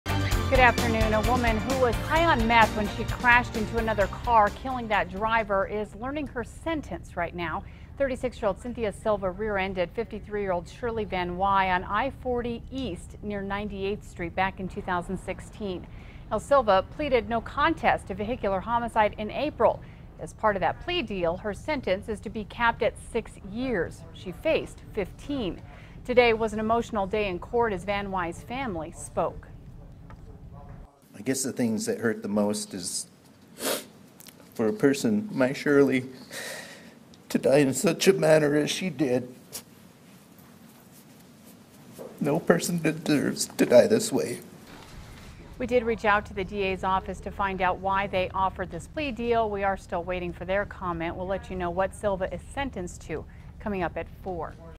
Woman accused of vehicular homicide expected to learn sentence Monday
Woman accused of vehicular homicide expected to learn sentence Monday - Source